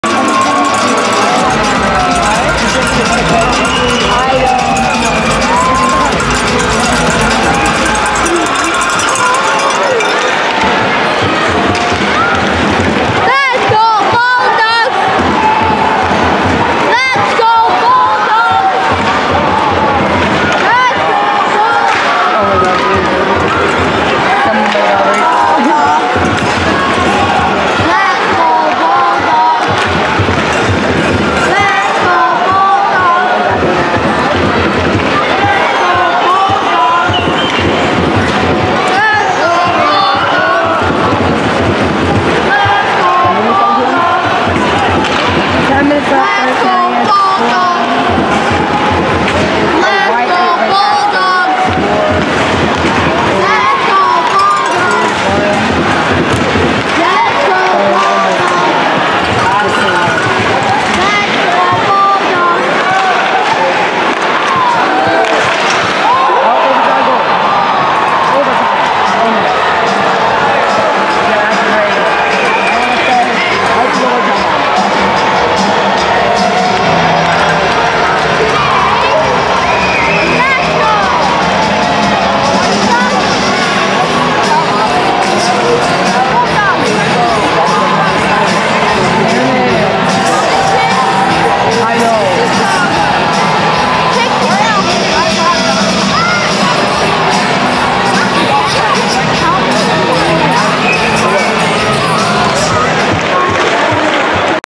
Steelheads VS Bulldogs - MISS 2 - 2 HAM (#7 Finn Harding) Ties The Game Up Late! (11/12/2022)
MISS 2 - 2 HAM

Steelheads Goal: #7 Finn Harding (2) Assists: #77 Ethan Del Mastro, #27 Zakary Lavoie.

With Just Less Than 10 Minutes Left Steelheads Player #7 Finn Harding Finds The Back Of The Net To Tie This One Late In The 3rd Period.